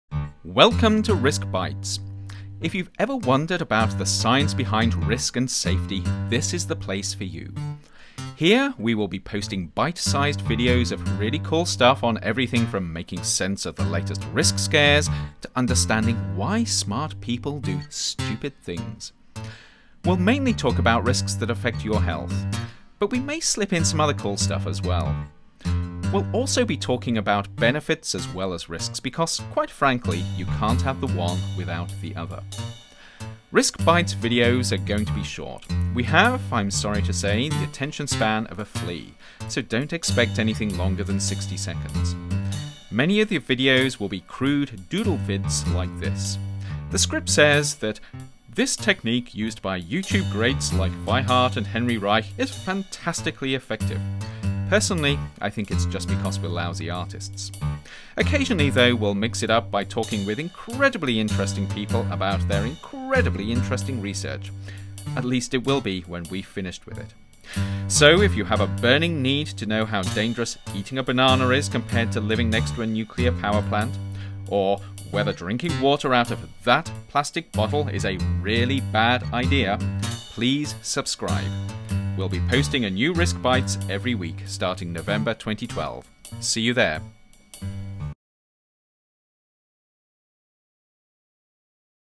Risk Bites Teaser
Coming November 2012 - bite-sized insights into the science or risk

For more information on this and other Risk Bites videos,